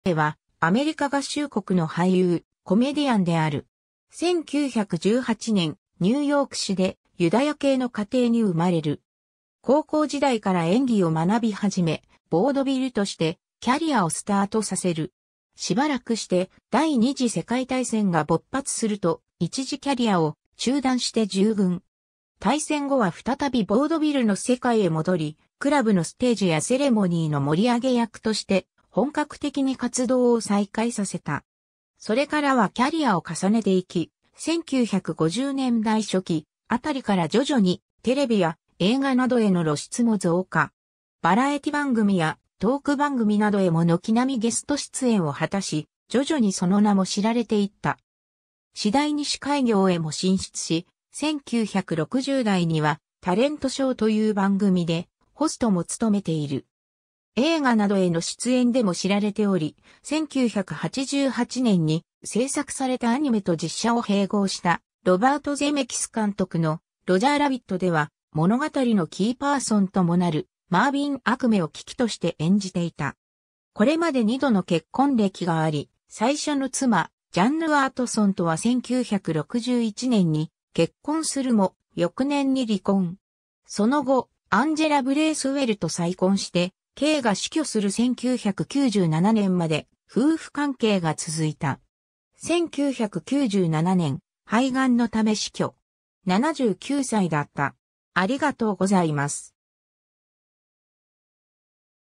スタッビー・ケイ
スタッビー・ケイ（Stubby Kaye, 1918年11月11日 - 1997年12月14日）は、アメリカ合衆国の俳優、コメディアンである。
 1918年、ニューヨーク市でユダヤ系の家庭に生まれる。
高校時代から演技を学び始め、ヴォードヴィルとしてキャリアをスタートさせる。
しばらくして第二次世界大戦が勃発すると、一時キャリアを中断して従軍。
大戦後は再びヴォードヴィルの世界へ戻り、クラブのステージやセレモニーの盛り上げ役として本格的に活動を再開させた。
それからはキャリアを重ねていき、1950年代初期あたりから徐々にテレビや映画などへの露出も増加。
バラエティ番組やトーク番組などへも軒並みゲスト出演を果たし、徐々にその名も知られていった。
次第に司会業へも進出し、1960代には「タレント・ショー」という番組でホストも務めている。
映画などへの出演でも知られており、1988年に製作されたアニメと実写を併合したロバート・ゼメキス監督の『ロジャー・ラビット』では、物語のキーパーソンともなるマーヴィン・アクメを喜々として演じていた。
 これまで二度の結婚歴があり、最初の妻Jeanne Watsonとは1961年に結婚するも、翌年に離婚。
 その後、アンジェラ・ブレイスウェルと再婚して、ケイが死去する1997年まで夫婦関係が続いた。
 1997年、肺がんのため死去。
79歳だった。